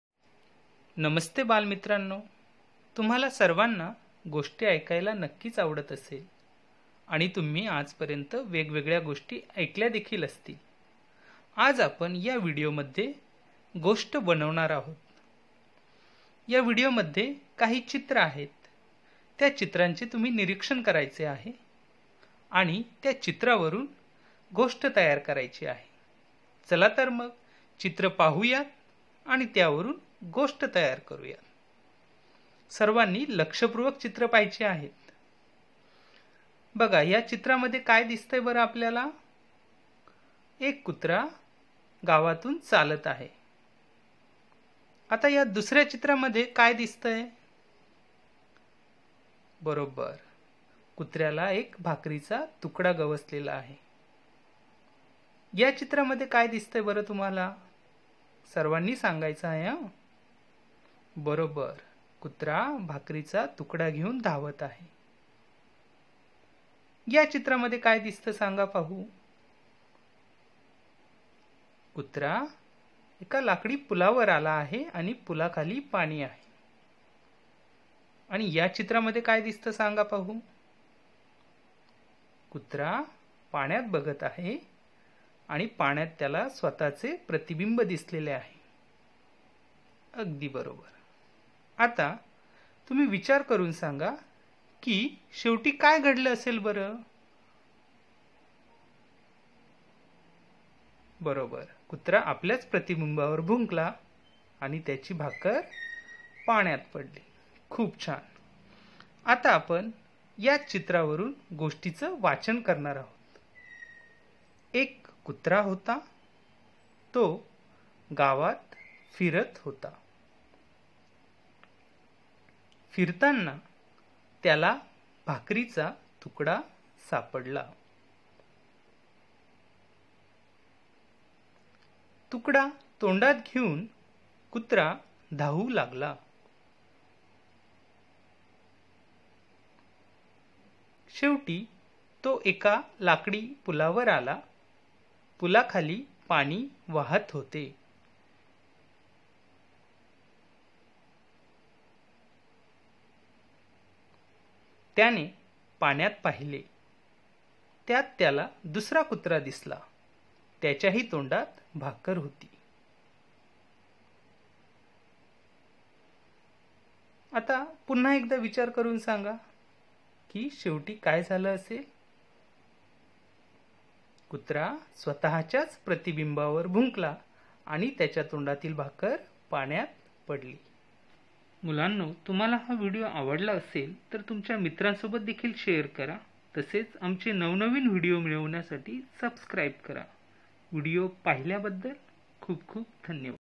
लोभी कुत्रा । Greedy dog । चित्रावरून गोष्ट तयार करणे ।
चित्रावरून गोष्ट तयार करा. गोष्ट सांगा व गोष्टीचे वाचन करा ।